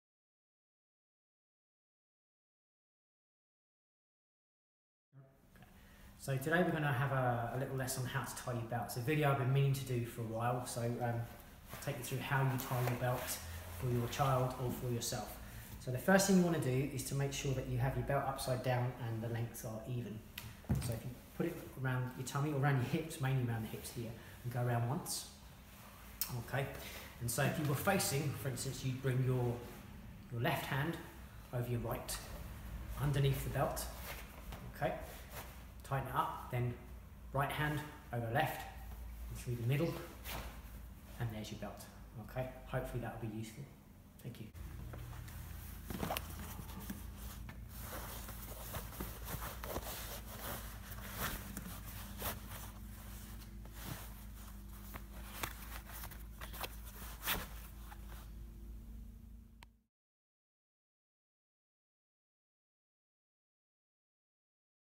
How to tie your karate belt
A guide for parents and students on how to tie a karate belt.